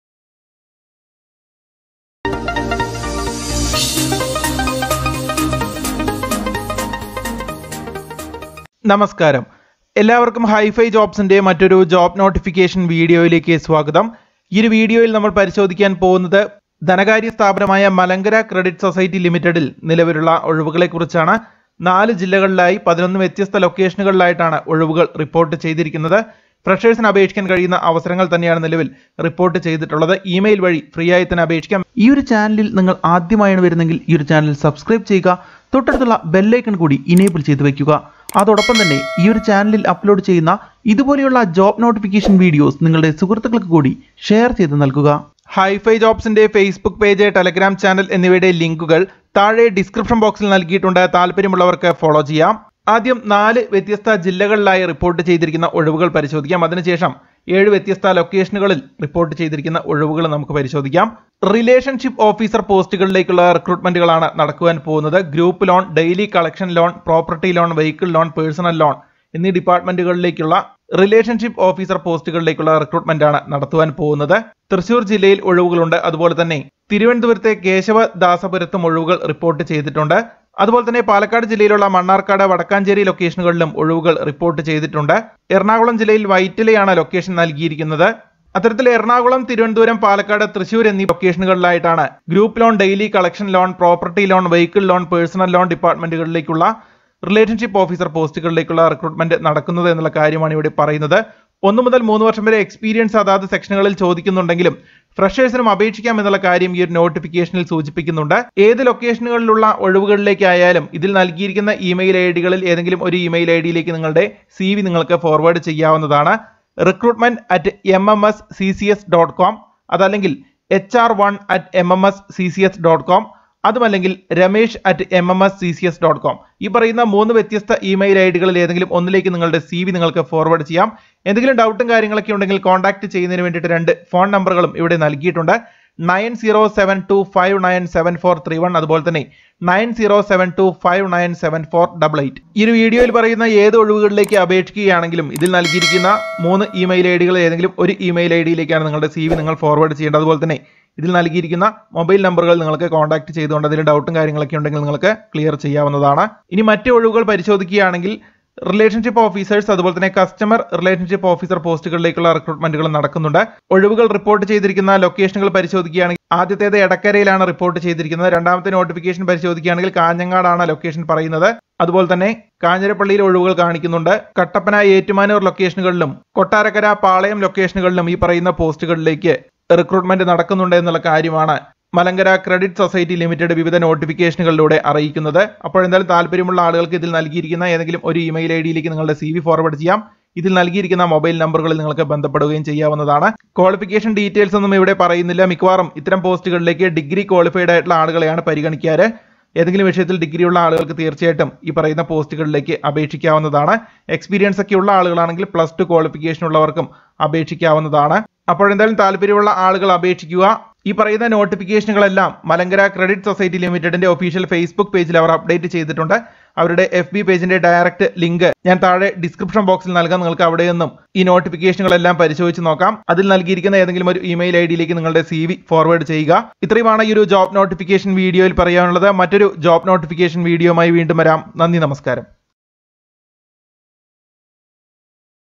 ക്രെഡിറ്റ്‌ സൊസൈറ്റിയില്‍ ജോലി നേടാം - Malankara Credit Society Recruitment 2023 - Bank Jobs 2023
Malankara Multi State Co Operative Credit Society Ltd invited applications from qualified candidates for the recruitment of various posts in Kerala.

😍🔷✅ Follow HiFi Jobs On Social Media ✅🔷😍